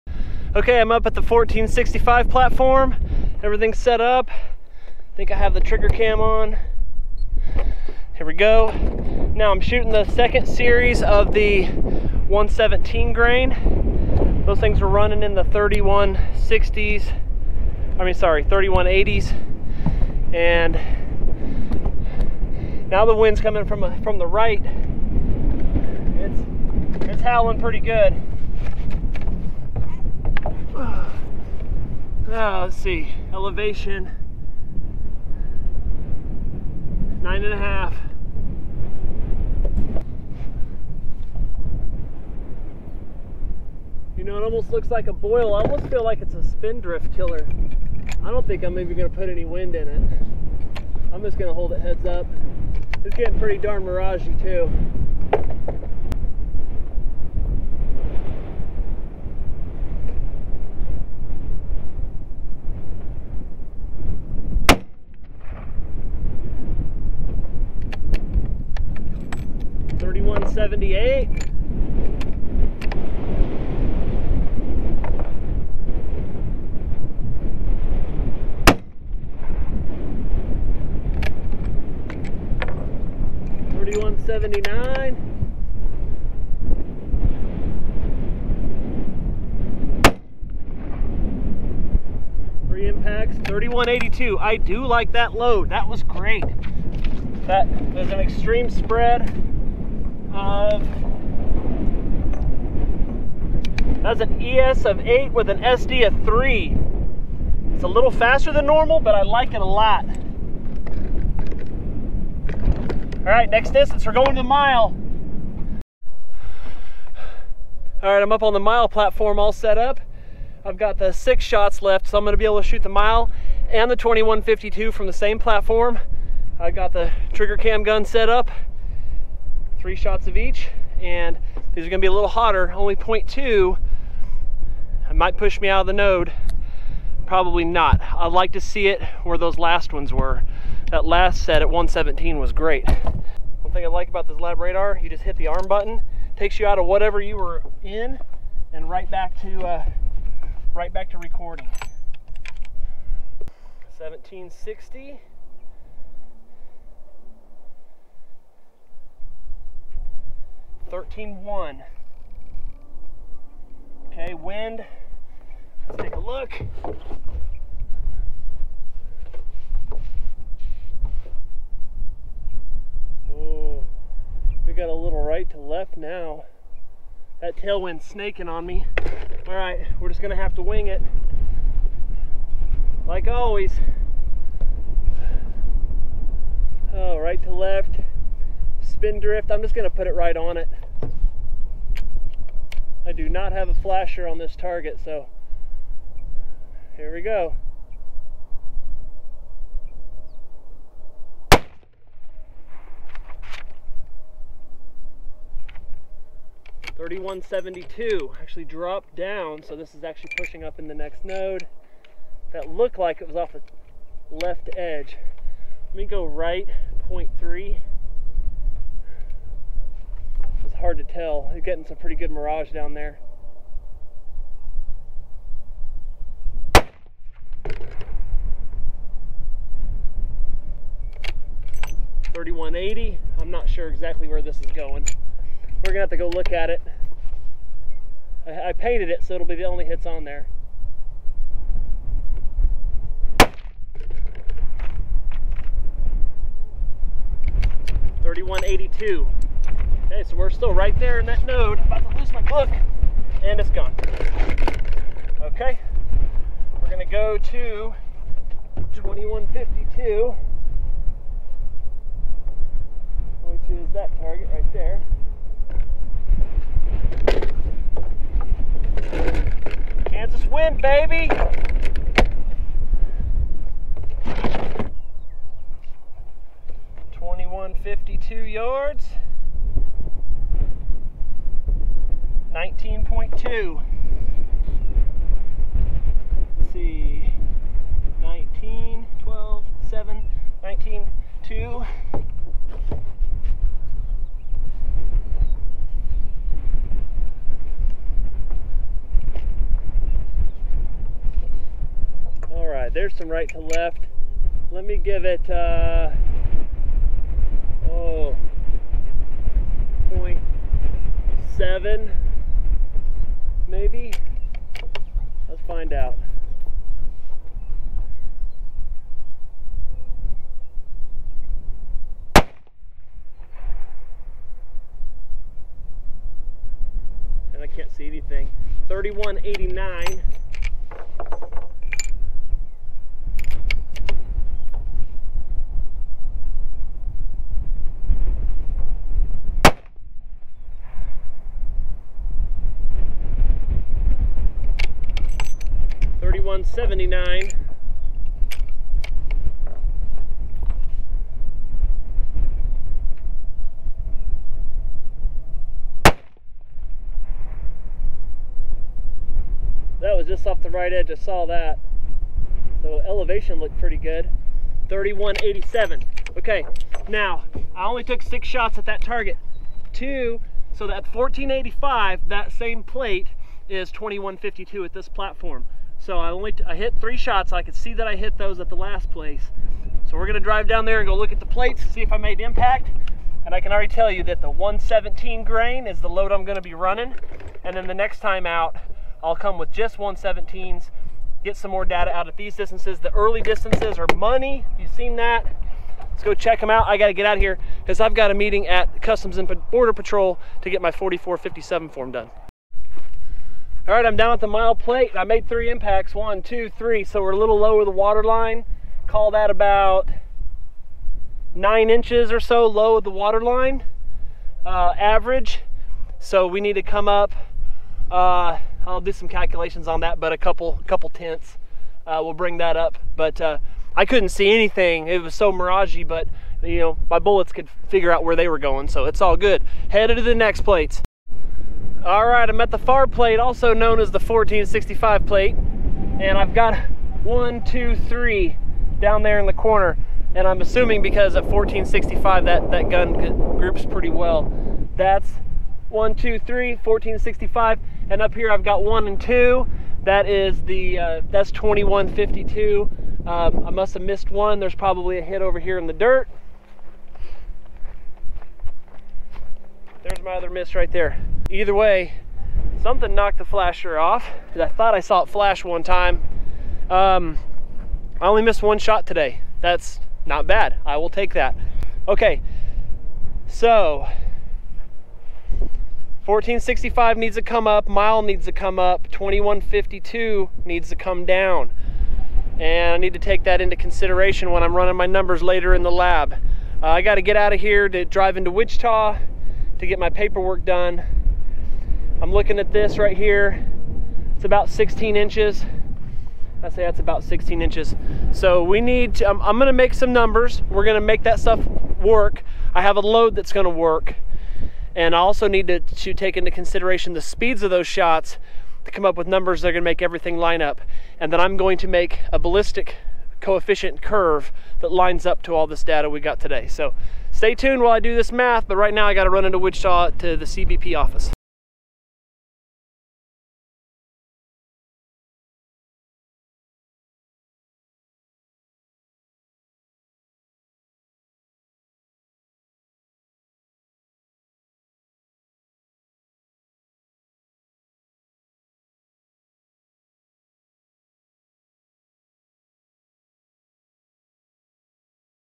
Giving away my secrets, load testing 33xc at 1465, 1760 and 2152 yds part 2 of 3
This is part 2 where I shoot the final 3 targets, 3 shots at each.  117gn of powder, then 117.2 twice.  Wind was getting pretty nasty and the mirage was pretty brutal, but all things considered, I got some great data.